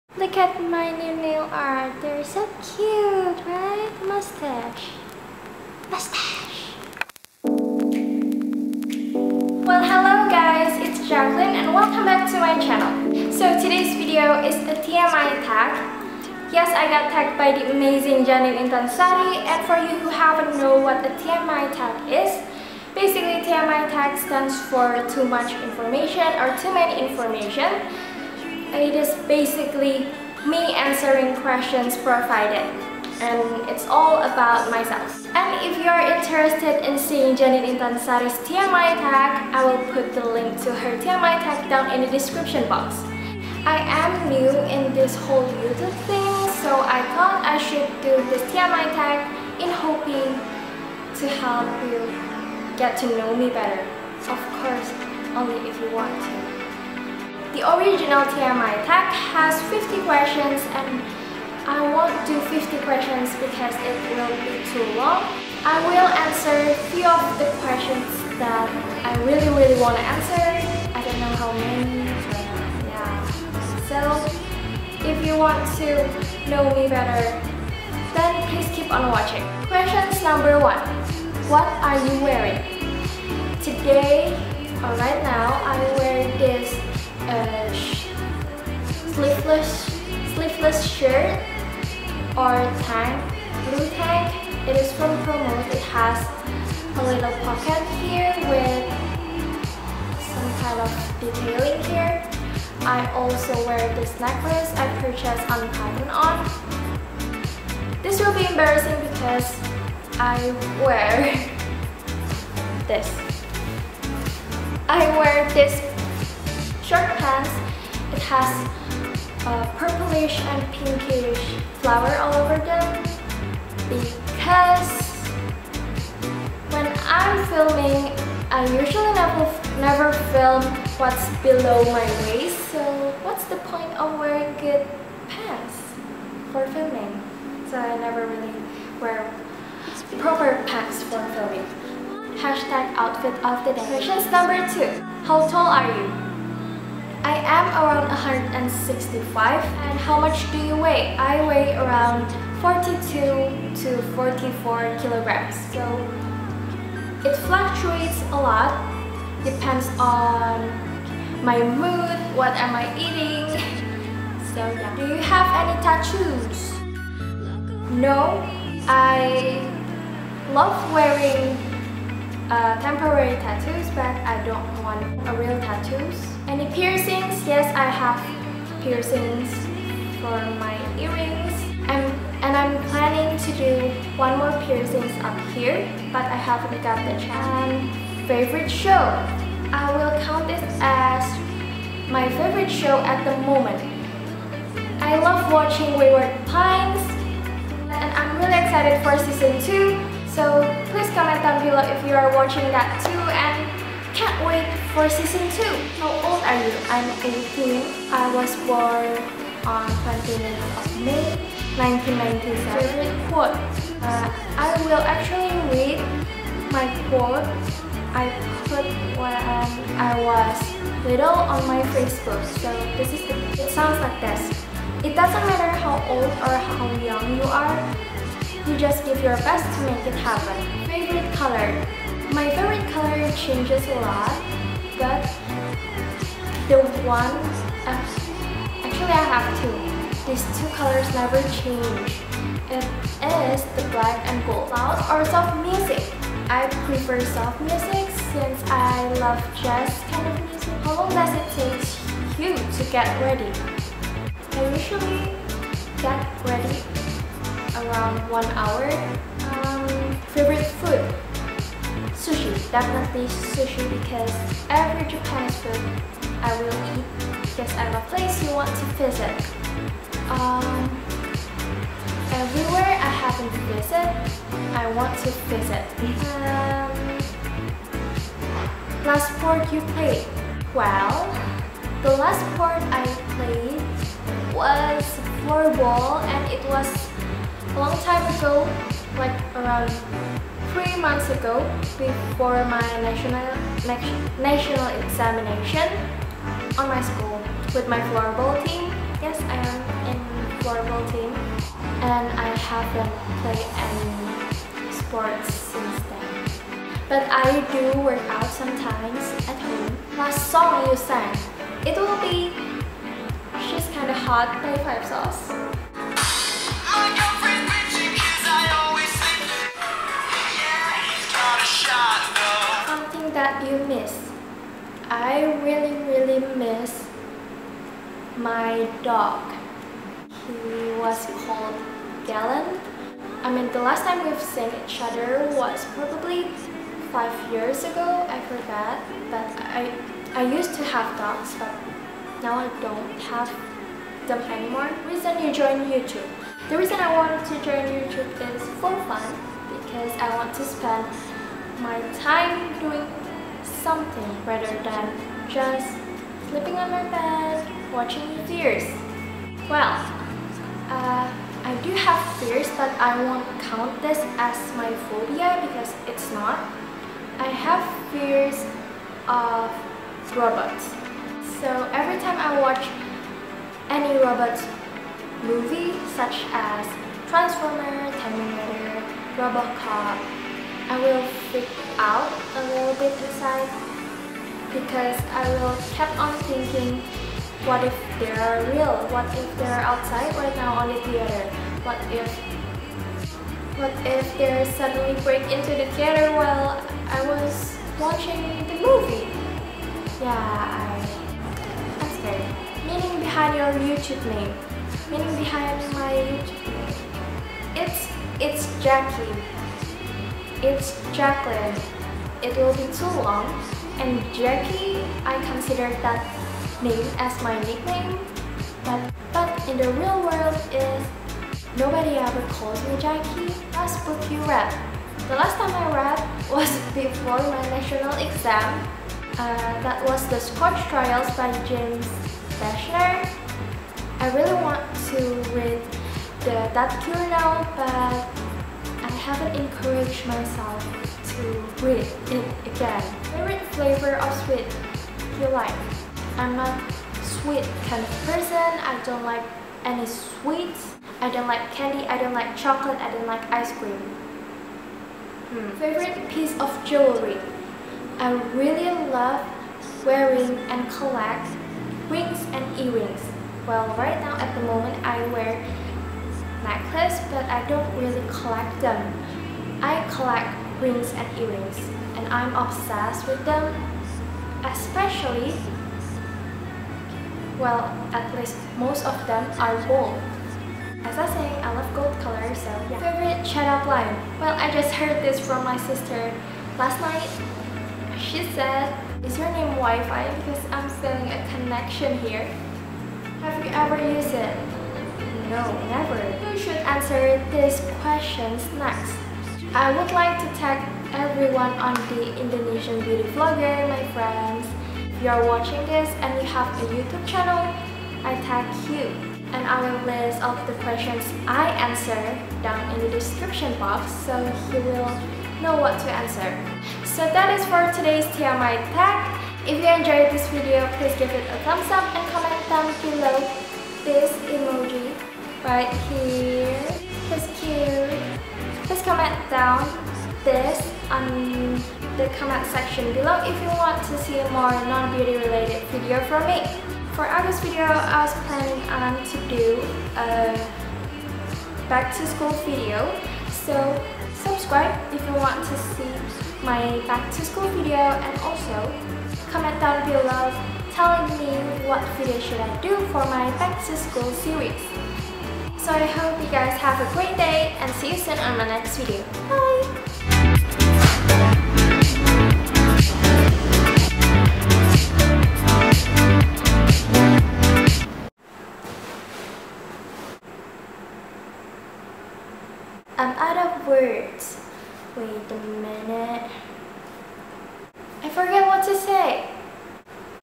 T.M.I TAG ! Fears, working out, youtube, etc. ! | ☛ itsJacquie !
Hey Peeps !  ´･ᴗ･`
Today's video is a non beauty related video. I'm doing the TMI TAG - TOO MUCH INFORMATION !
i got tagged by the amazing JANINE INSTANSARI ! Here i am answering some of the questions like my fears, youtube, favorite stuff, and many more- basically everything you need to know about me !
so, KEEP ON WATCHING IF U WANT TO GET TO KNOW ME BETTER !

\ J A N I N E ' S   T M I T A G \

1. WHAT ARE YOU WEARING ?
2. HOW TALL ARE YOU ?
3. HOW MUCH DO YOU WEIGH ?
4. ANY TATTOOS ?
5. ANY PIERCINGS ?
6. FAVORITE SHOW ?
7. HOW OLD ARE YOU ?
8.FAVOURITE COLOR ?
9. LOUD / SOFT MUSIC ?
10. HOW LONG DOES IT TAKES FOR YOU TO GET READY ?
11. FAVORITE FOOD ?
12. PLACES YOU WANT TO VISIT
13. LAST SPORT YOU PLAYED
14. LAST SONG YOU SANG
15. SOMETHING THAT YOU MISS
16. REASON YOU JOIN YOUTUBE
17. FEARS
18. MEANING BEHIND YOUR YOUTUBE NAME
19. LAST BOOK YOU READ
20. FAVORITE FLAVORS OF SWEET
21. FAVORITE PIECE OF JEWELRY
22. FAVORITE CHAT UP LINE
23. HAVE YOU EVER USED IT ?
24. WHO SHOULD ANSWER THIS QUESTIONS NEXT ?

\ S O C I A L N E T W O R K I N G \

Thanks for spending your time,

      love you peeps ! ♥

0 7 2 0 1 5